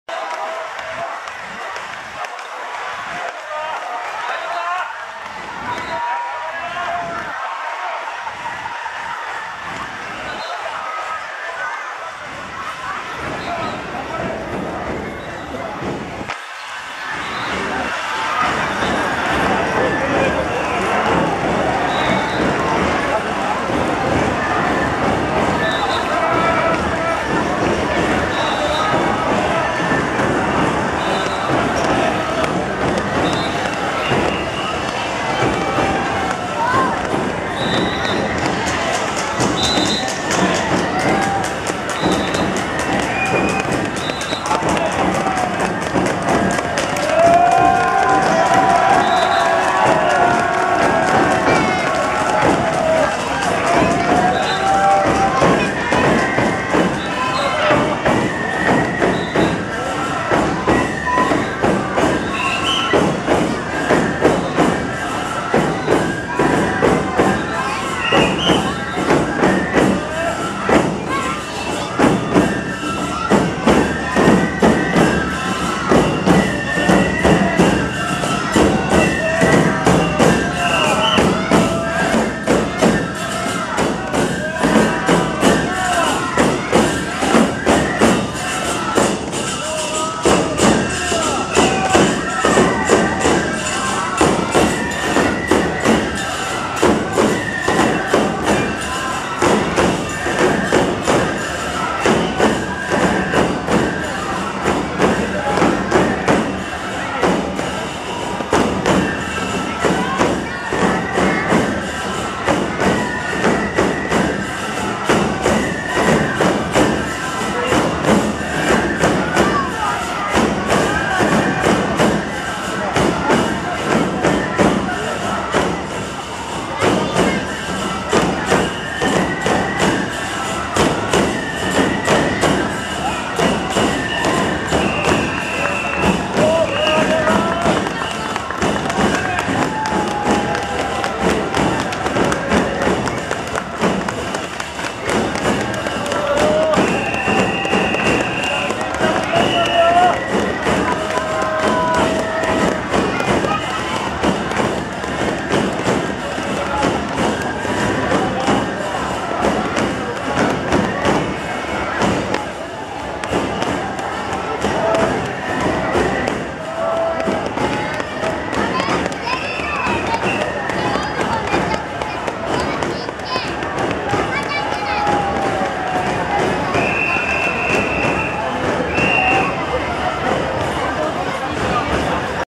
青森ねぶた祭り２０１３年－１４
２０１３年８月５日の青森ねぶた祭り
東北三大祭りの風景は素晴らしい